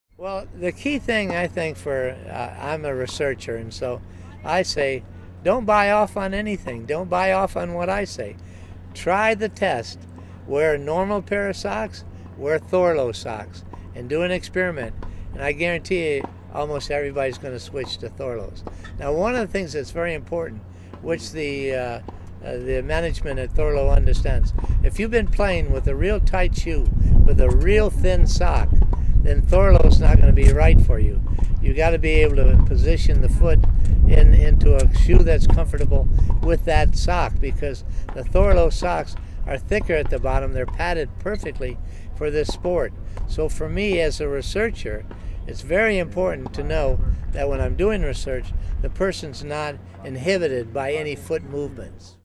Thorlos : The Best Tennis Sock : Proper Fitting
Vic Braden discusses why Thorlos are the best tennis sock.